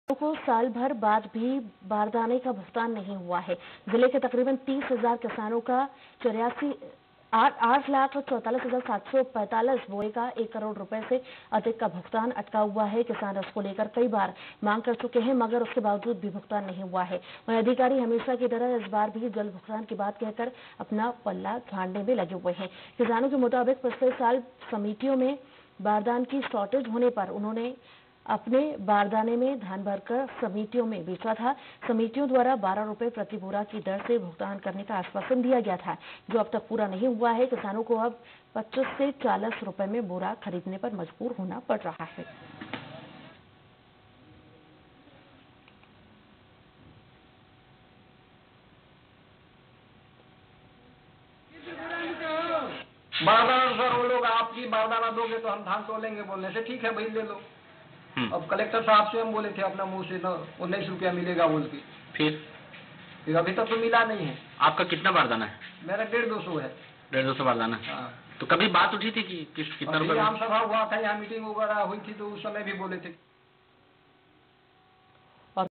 Gariyaband News: सालभर बाद भी नही हुआ किसानों को बारदाने का भुगतान
Gariyaband News: सालभर बाद भी नही हुआ किसानों को बारदाने का भुगतान GariyabandNews Chhattisgarh farmers Gariyaband District Purushottam Patra